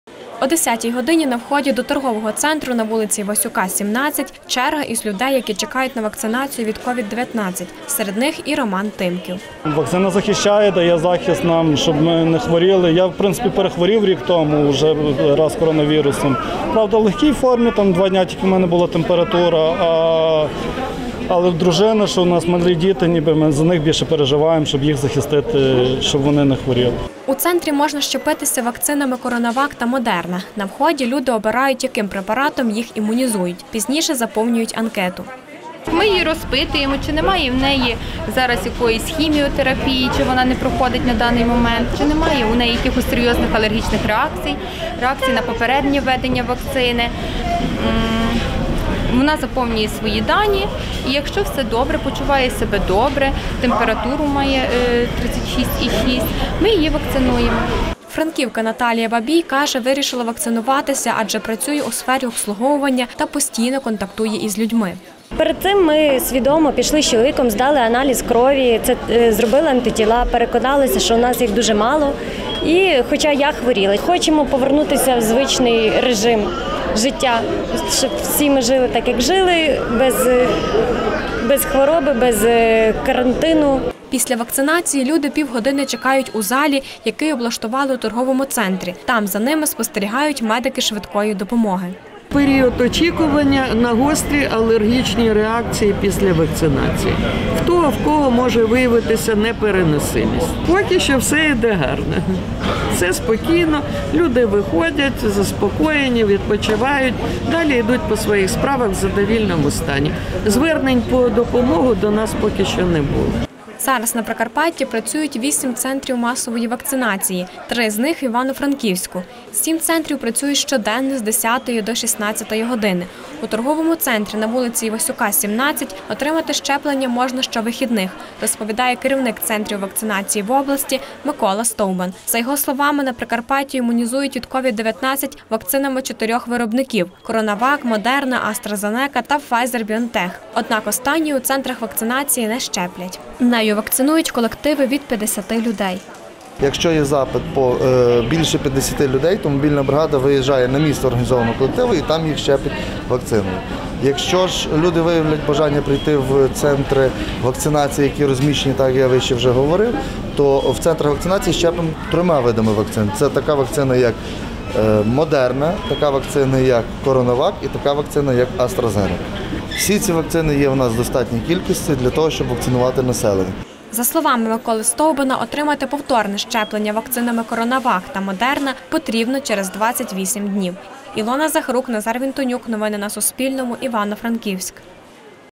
Як у франківському "Епіцентрі" проходить вакцинація від COVID-19
31 липня в Івано-Франківську в "Епіцентрі" запрацював центр вакцинації від COVID-19. Він розташований на вулиці Івасюка, 17. Щепитися там можна щовихідних з 10-ї до 16-ї години. Про те, як у центрі проходить імунізація, дивіться у відео.